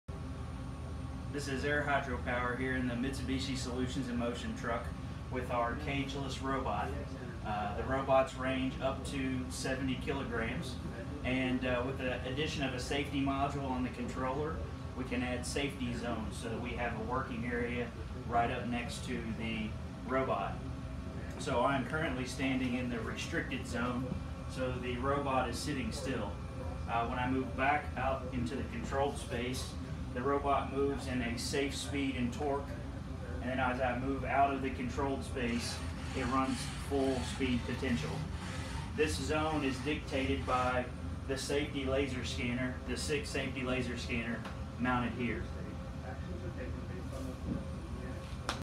Mitsubishi RV-F Robot Video
Cage free robot utilizes safety laser scanner for safety zone.